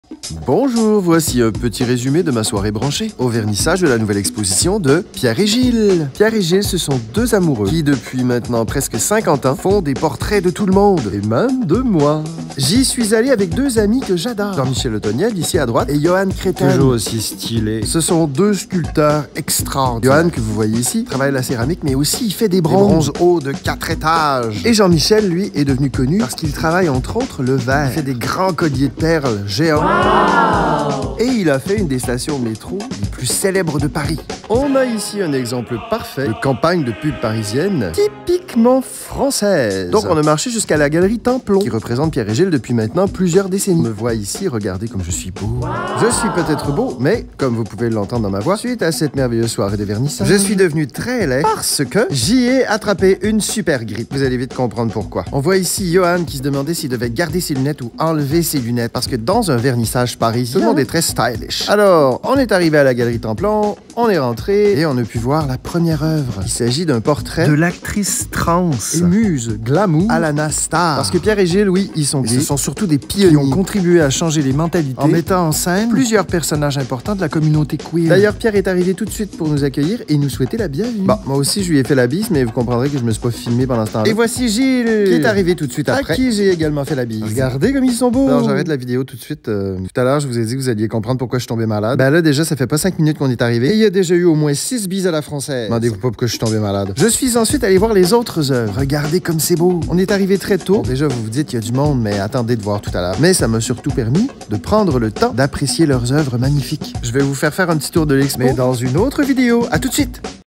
1/2 Vernissage de Pierre et Gilles à Paris
Le 14 septembre dernier, je suis allé au vernissage de mes amis Pierre et Gilles à Paris. Voici la première vidéo qui résume ma soirée, la deuxième arrive sous peu ;)